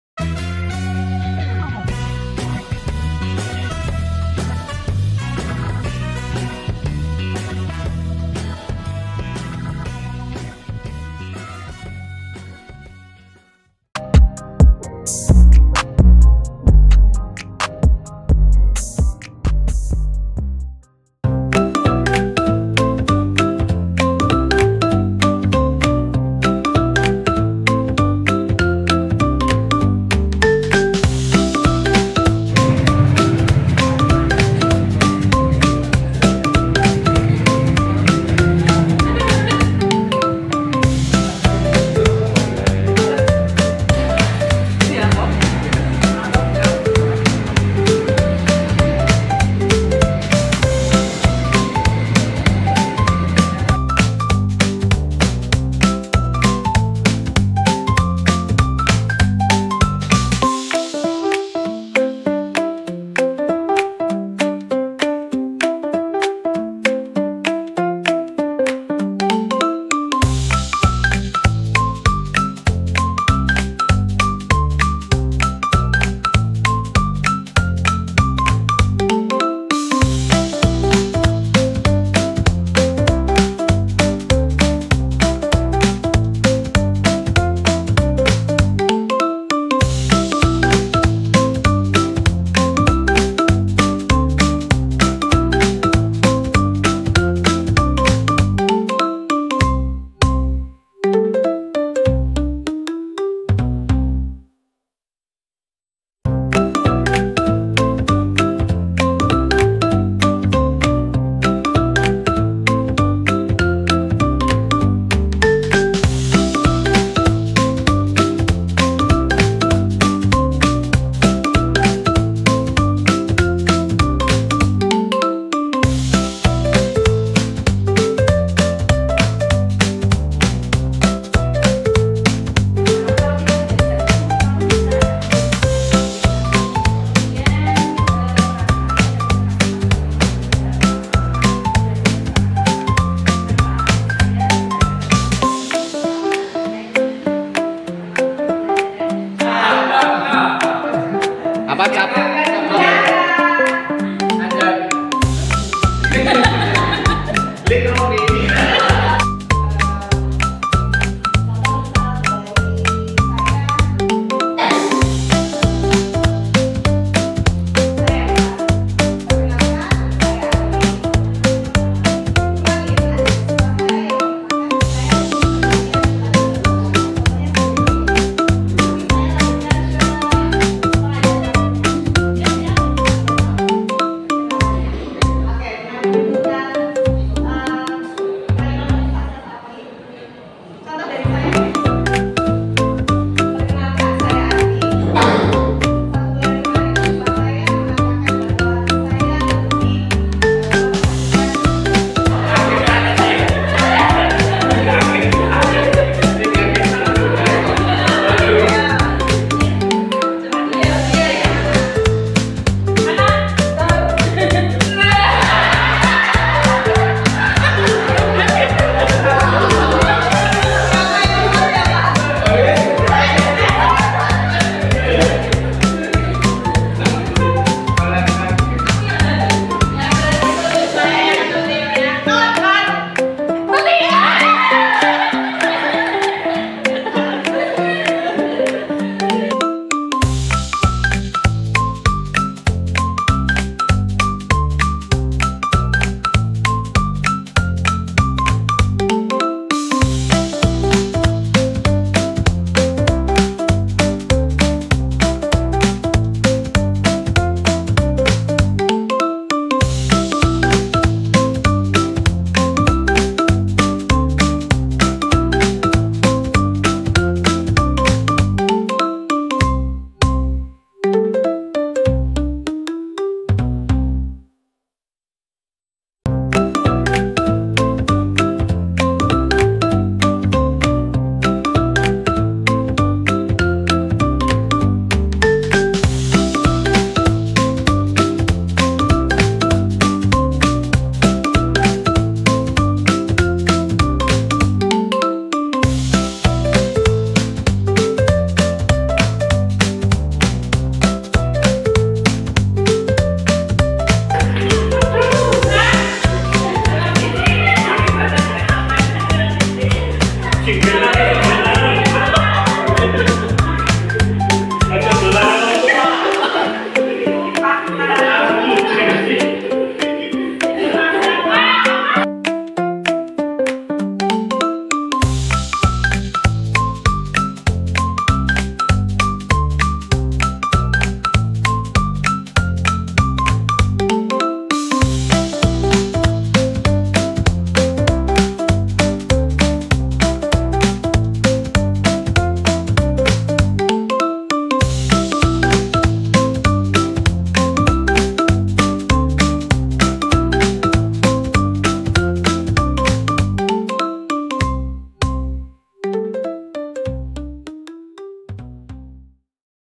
Lokakarya 1 Calon Guru Pengeerak A.10
Lokakarya 1 terdiri dari 3 kelompok , bertempat di  SMK Muhammadiyah 1 Purbalingga hari Minggu, 21 Juli 2024 pkl. 07.30 s/d 16.00 WIB